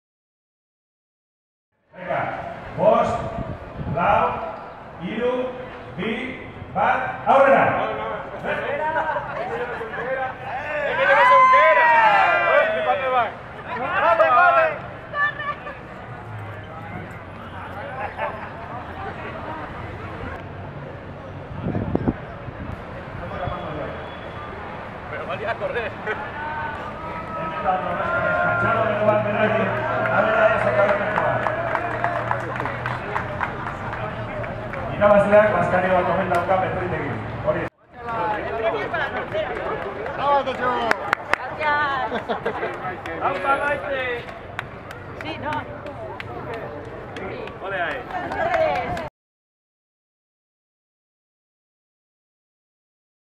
Zerbitzari lasterketa Amarako jaietan
Aurten ere zerbitzari lasterketa egin dute Easo plazan, Amarako jaien egitarauaren barruan. Hiru ausartek parte hartu dute.